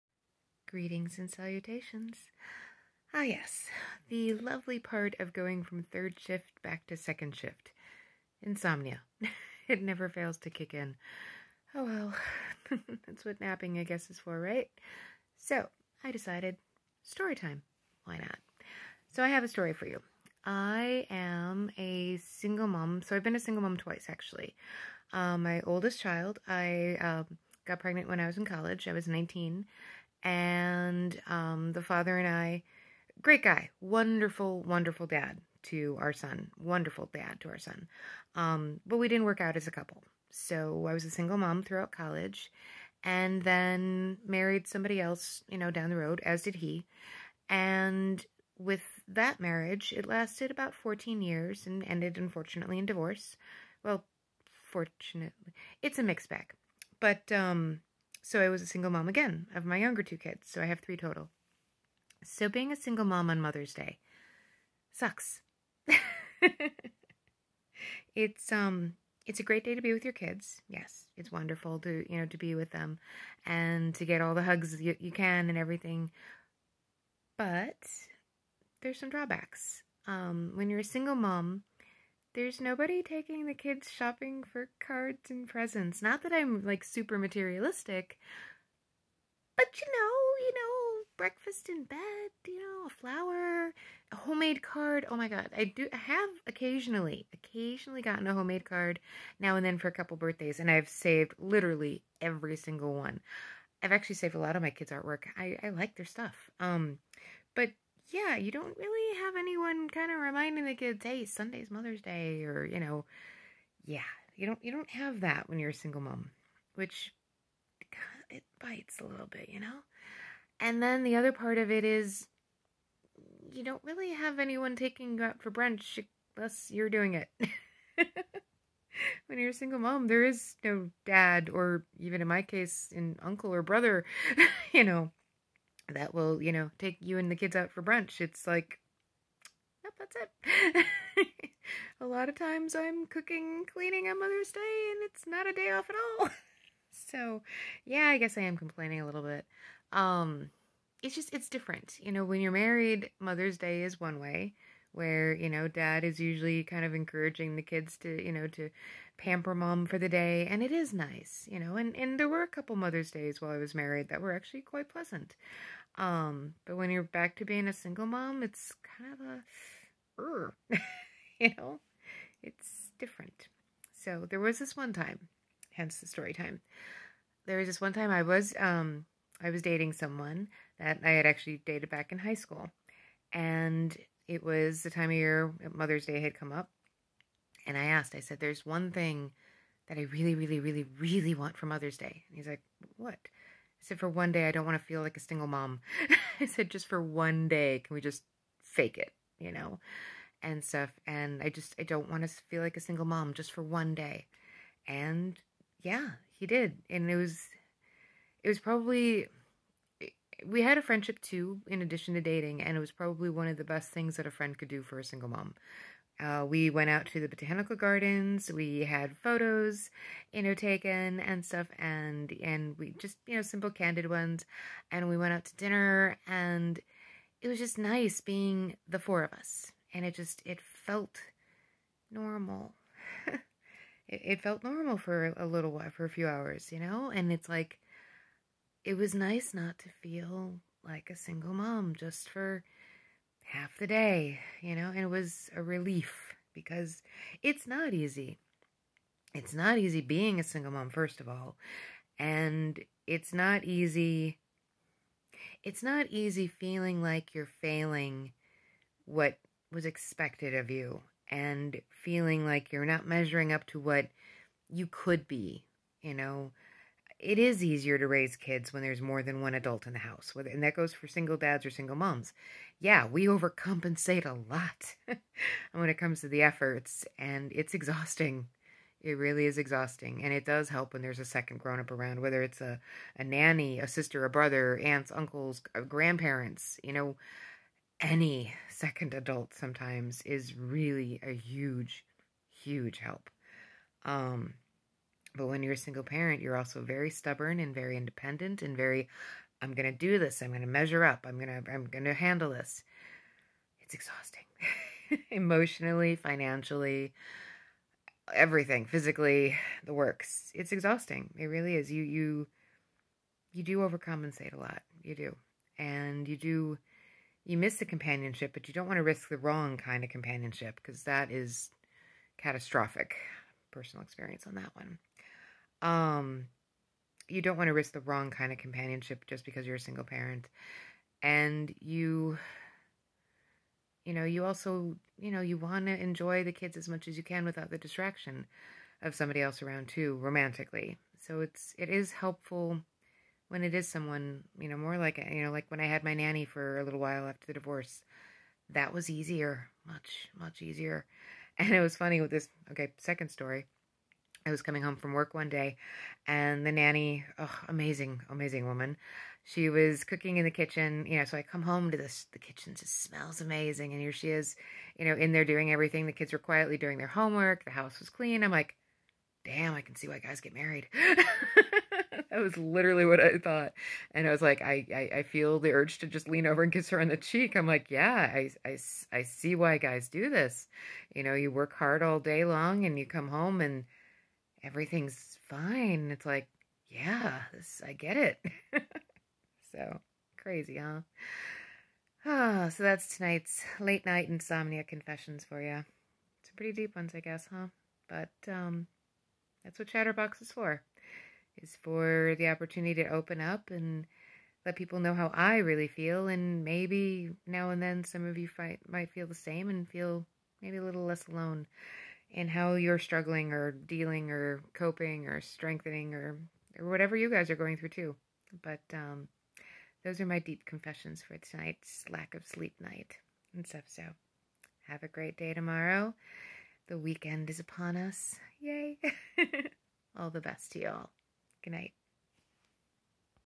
Chatterbox - late night ponderings
Sharing a perspective on mother's day as a single mom